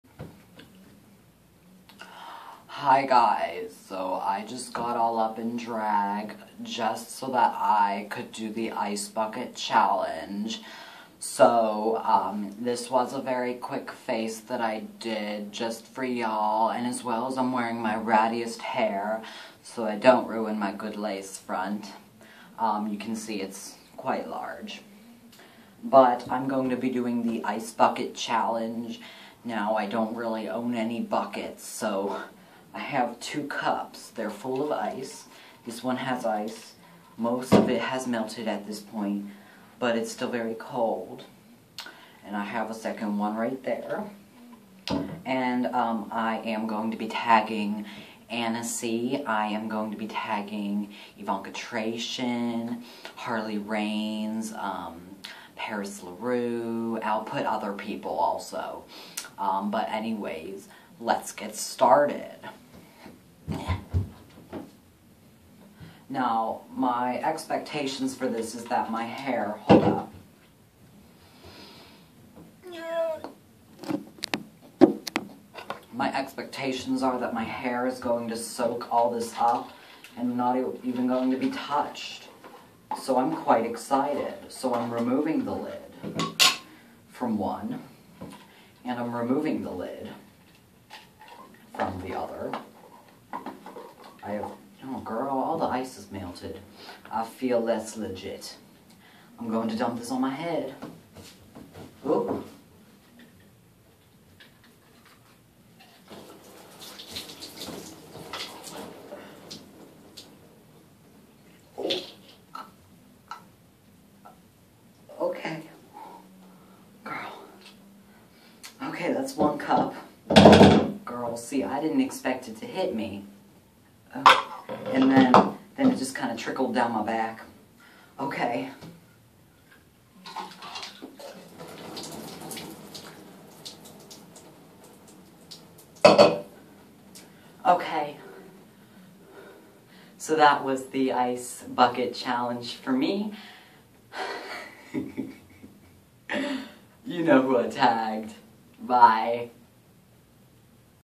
Teen Drag Queen Does the Ice Bucket Challenge!
I tag Annecy, Evonka, Paris, Harlee, Siren, and Emma!